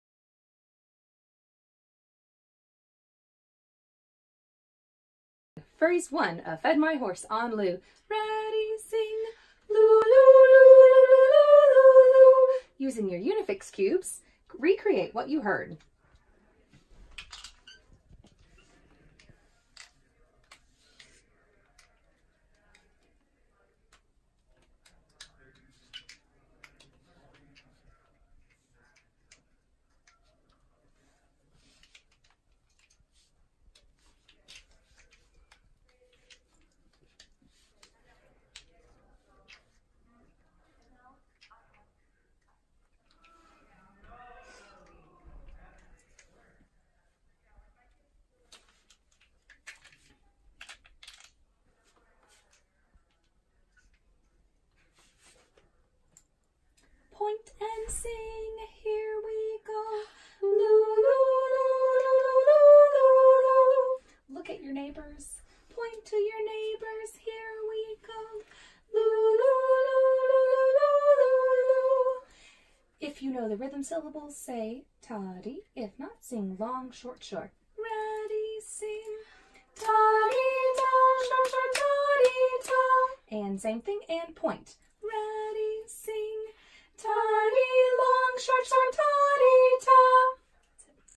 How to Teach Third Grade Music– Tadimi Part 3 (Visual)
Sequence of teaching events: Review kinesthetic and visual lessons, sing focus phrase on “loo” and keep a beat, students create visuals of the focus phrase, students sing and point to their visuals

This sequence of teaching can be found in Kodály in the Third Grade Classroom from the Kodály Today handbook series by Mícheál Houlahan and Philip Tacka, pages 57-64, and pages 188-190.

Kodály Today

Kodaly in the Third Grade Classroom: